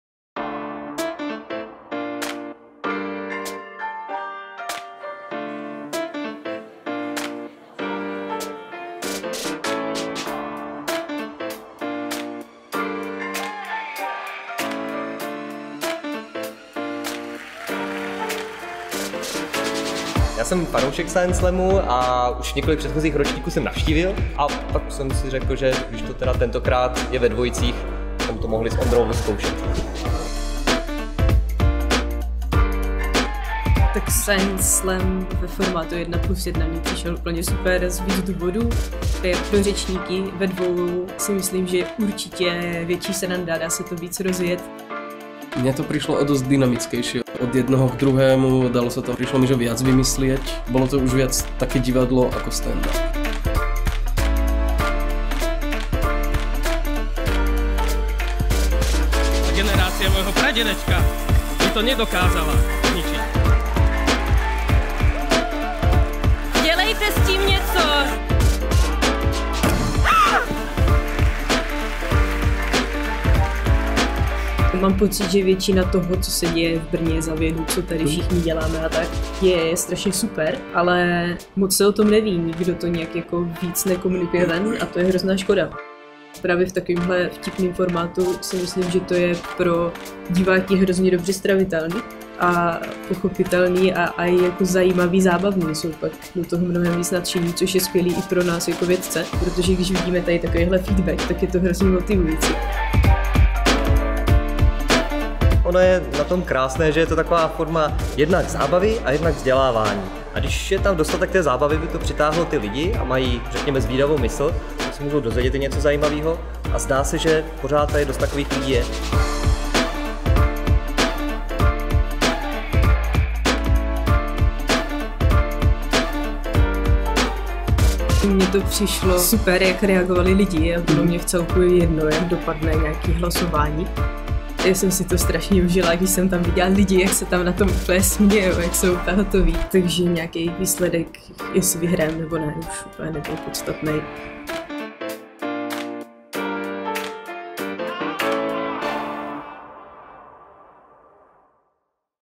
Science slam 11: Jaké to bylo
Science slam 11 proběhl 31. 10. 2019 v prostorách Univerzitního kina Scala. Poprvé se na pódiu vystřídaly dvojice vědců.